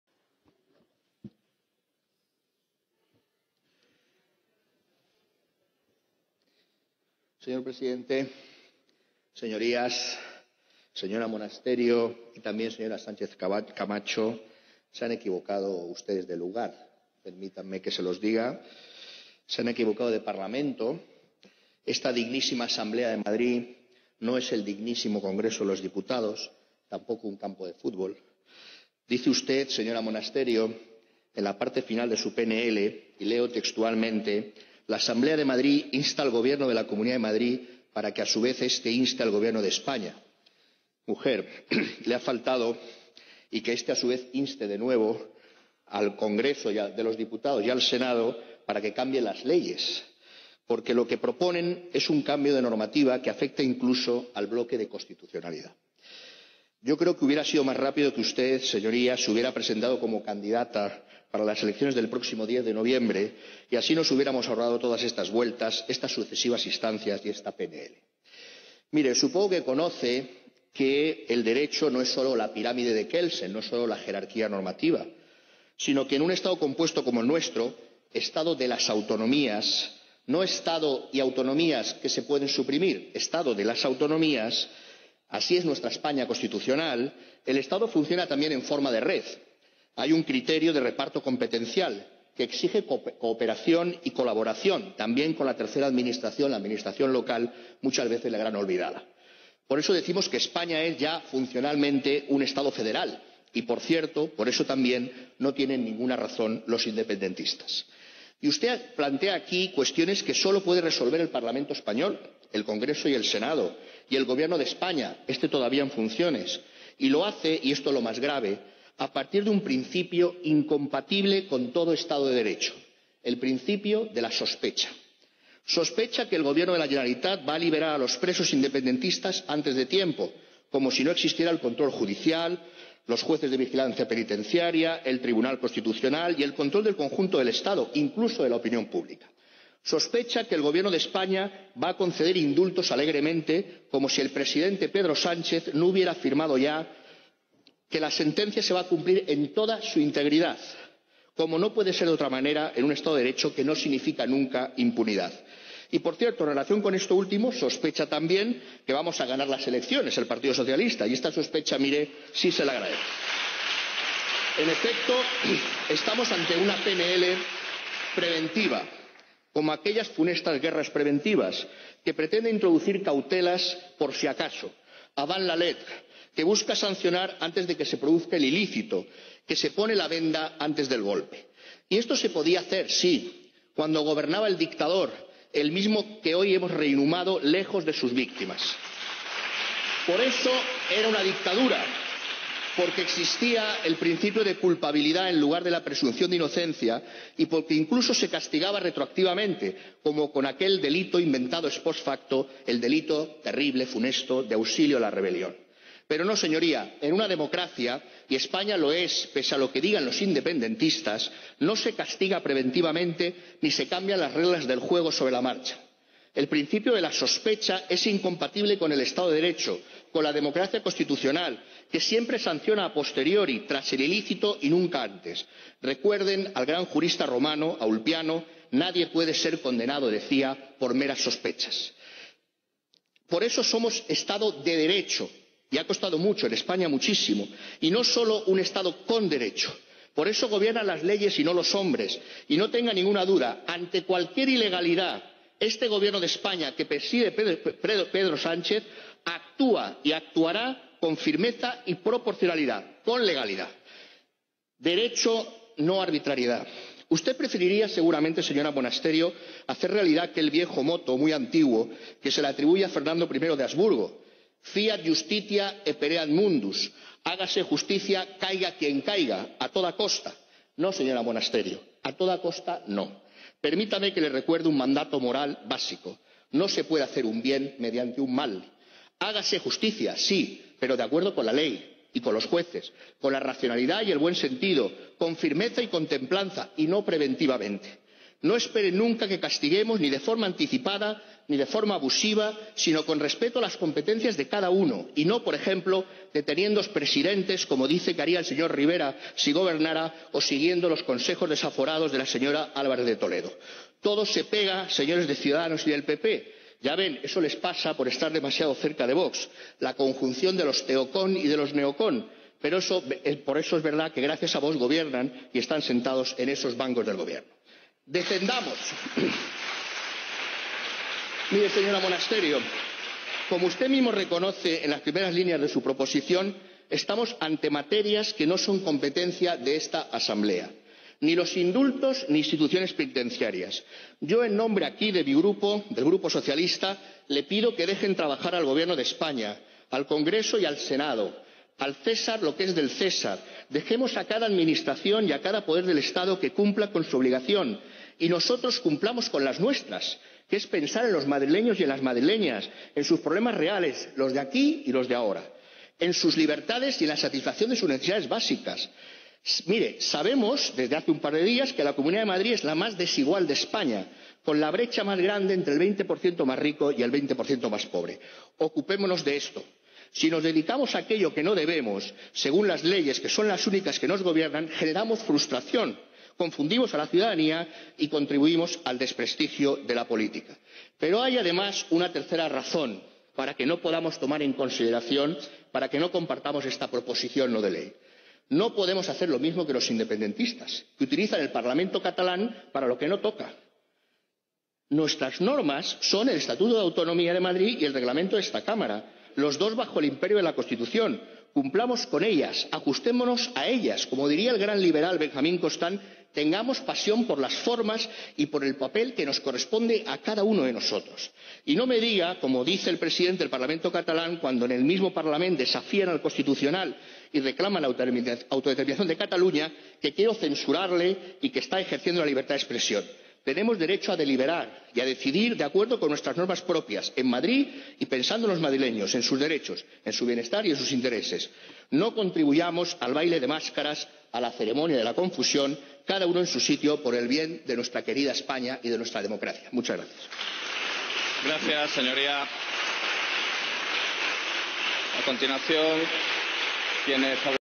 Uribes: "En una democracia, y España lo es, no se castiga preventivamente"
Pleno de la Asamblea de Madrid